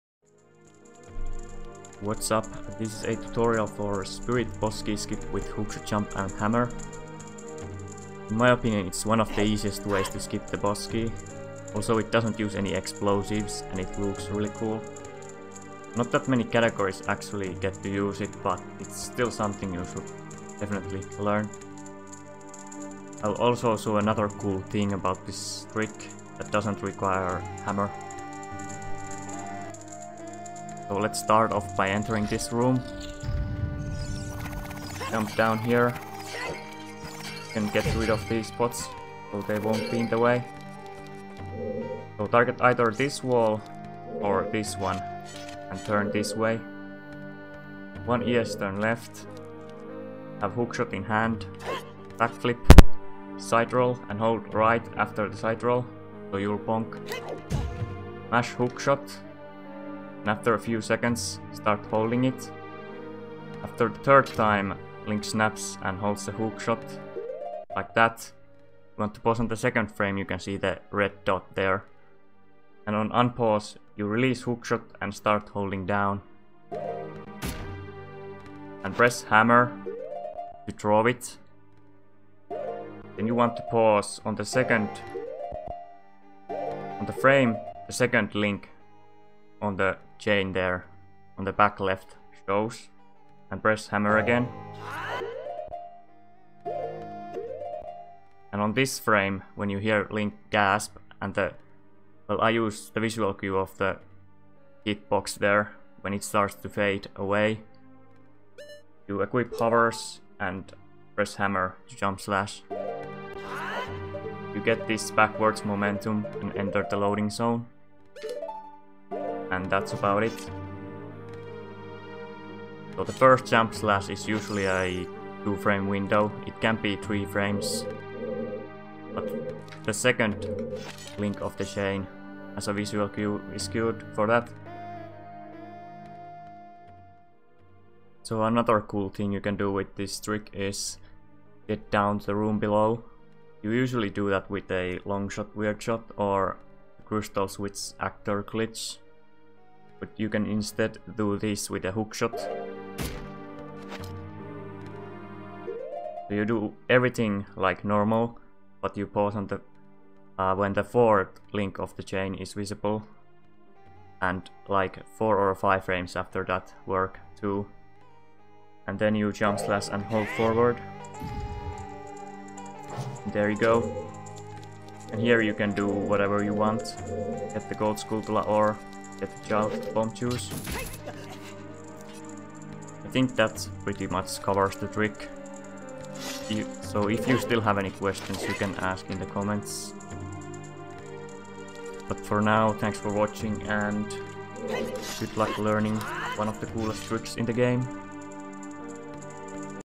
Spirit BK skip with Hammer & Hookshot Jump Tutorial
Hookshot jump method with bonking was found by rosewater, setup is originally by skater but I couldn't find the video, you should check exodus video on this trick too. I'm not sure if someone else found out about entering the room below might have been bakeneko so shoutouts to these guys lol.